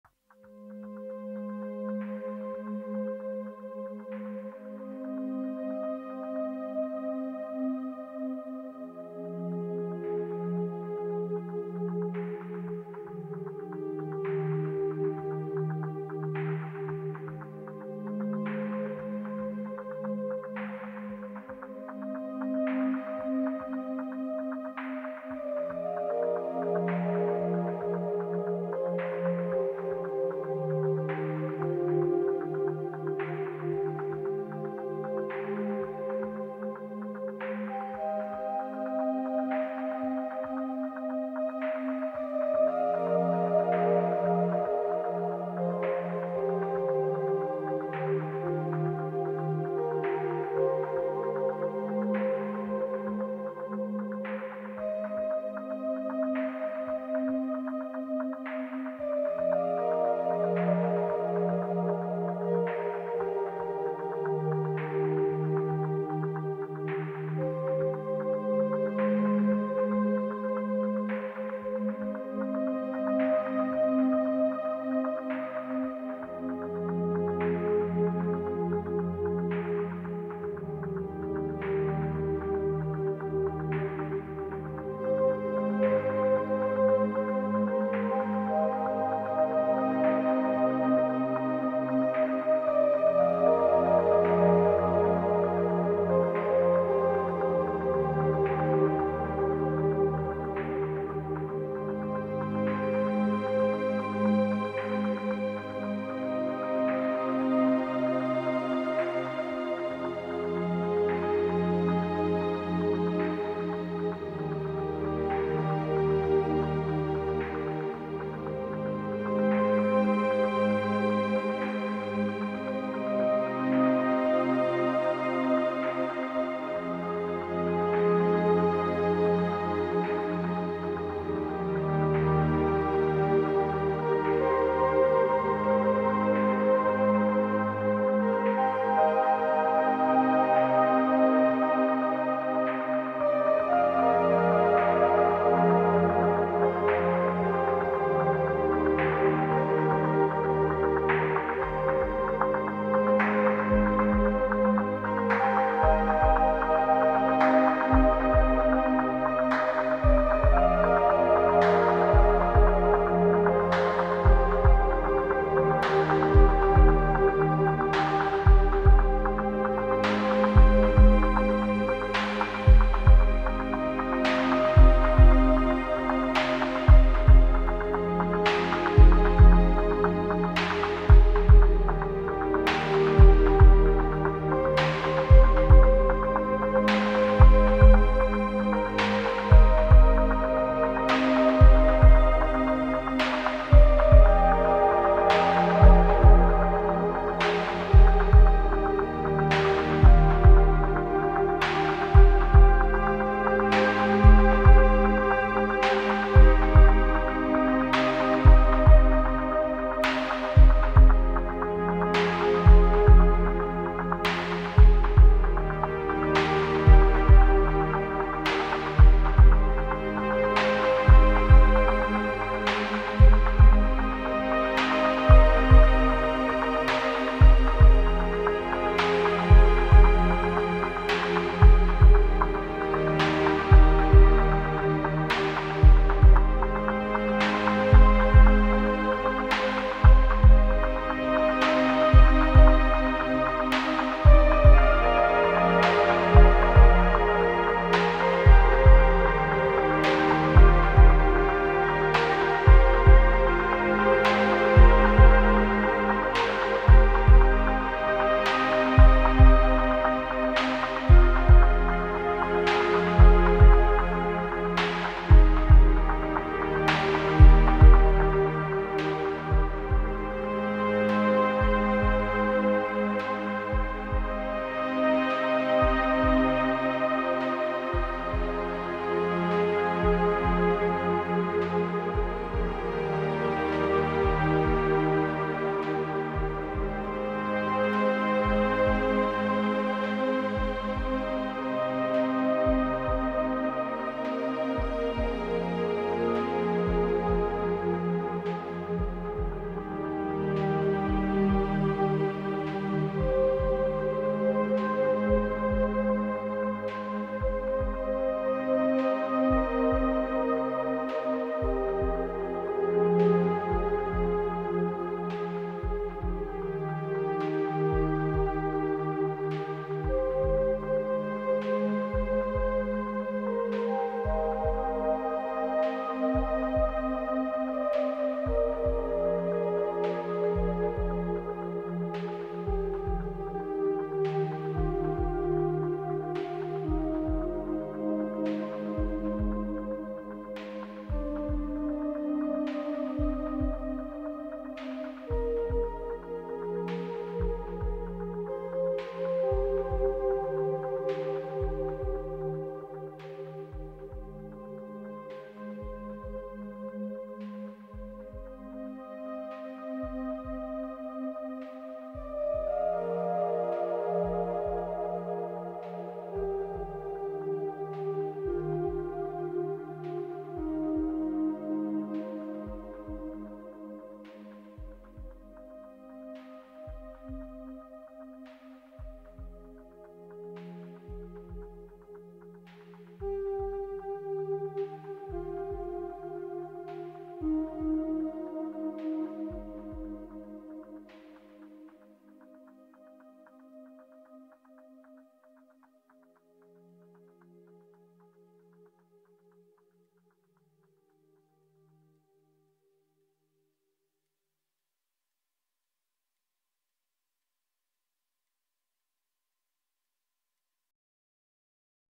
Live cinematic ambience // Microfreak, Minilogue, Olafur Arnalds chamber evolutions
Really enjoyed recording this track live ..its got quite a cinematic feel to it. With the help of the lovely Valhalla shimmer I was able to get some real nice sounds from both of the synthesizers. Its all improvisation so i am aware it is far from perfect! but anyway thanks for spending some time listening to it and i hope you like it.